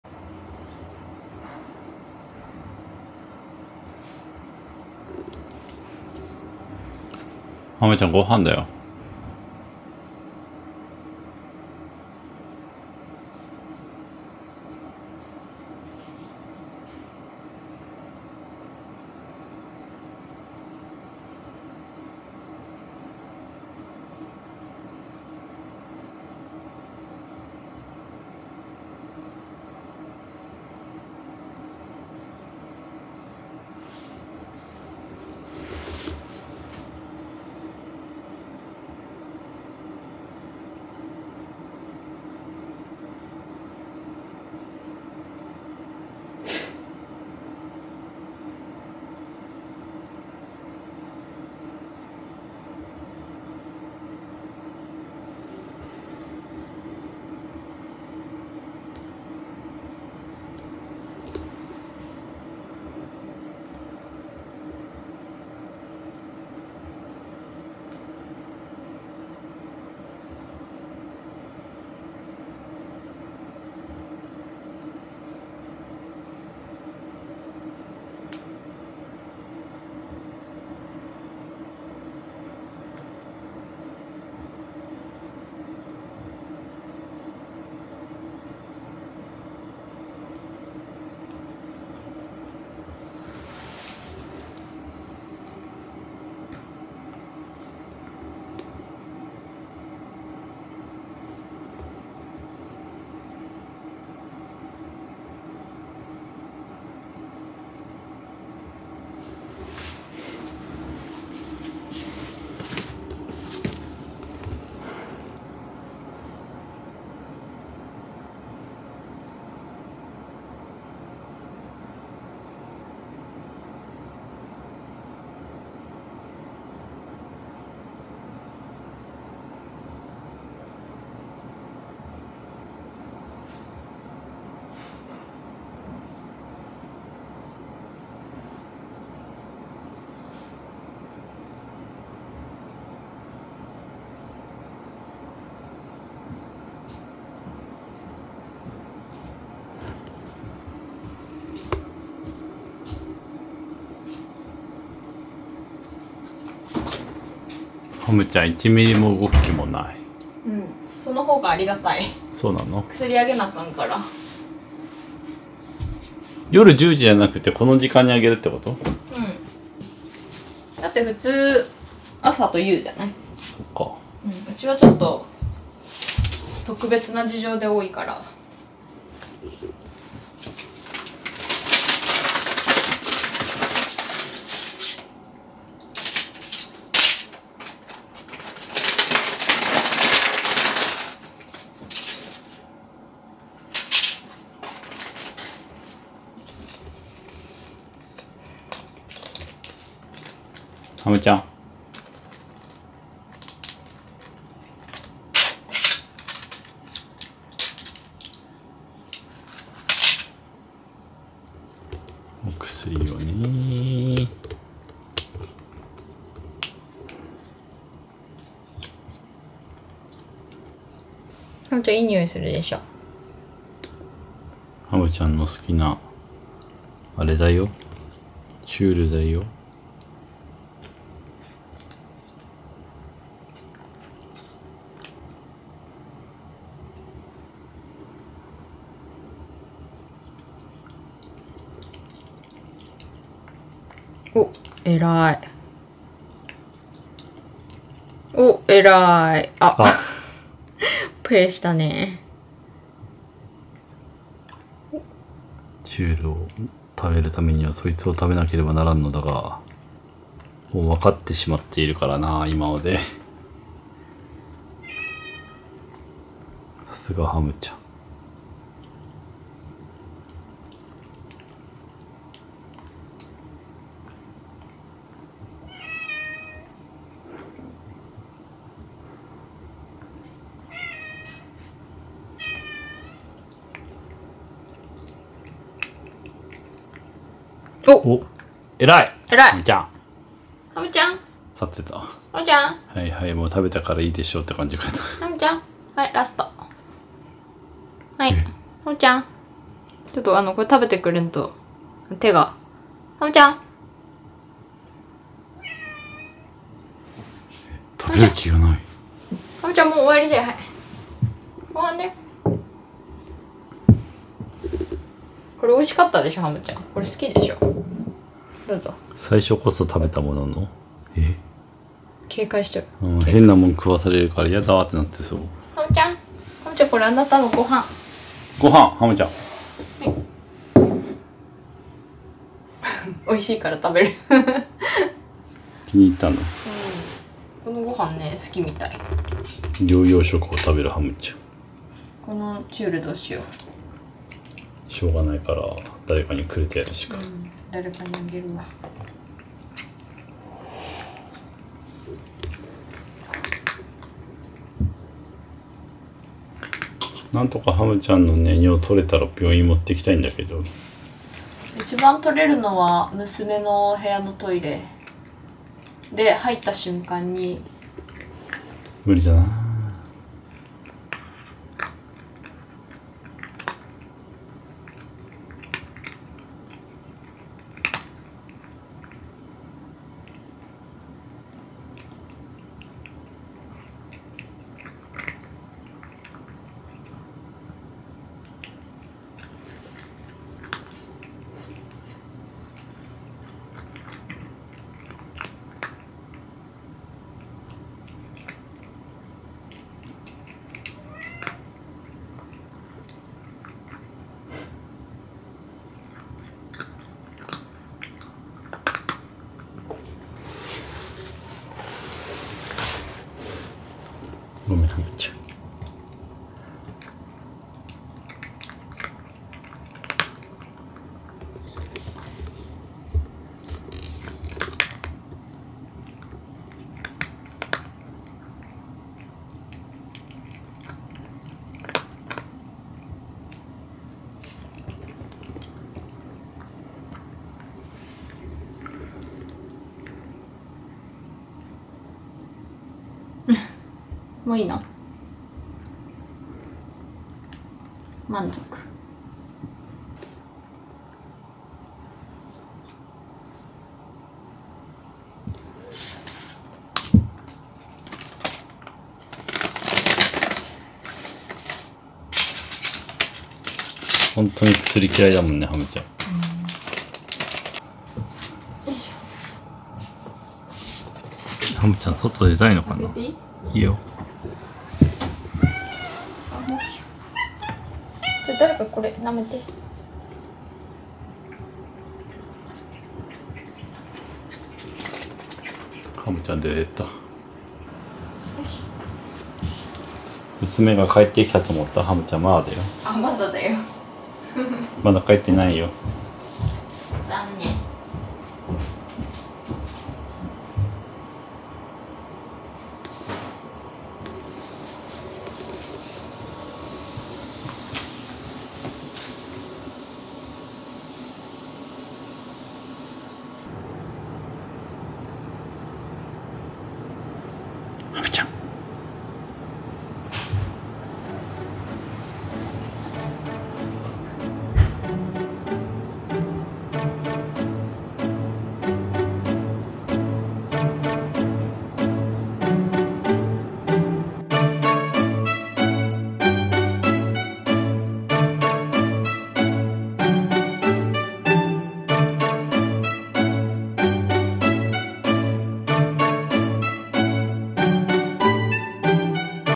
膀胱炎のお薬を嫌がる猫がごはんも避けるようになってきた…
やっぱり病気はさせるもんじゃないなぁと思ったとか何とか…。

ハムはお薬が大嫌いなのでこの時はめちゃくちゃ嫌がってまして…やっぱり変化が嫌いみたいで、ごはんを避けたり、チュールすら食べきらなかったり…。

ちなみに翌日には慣れたみたいでお薬もぺろーんと食べてましたのでご安心くださいｗ

撮影機材：
【SONY ZV-E1】

【ソニーシューティンググリップ】

サブ機種
【iPhone14ProMax】

【外部マイク】
アマゾン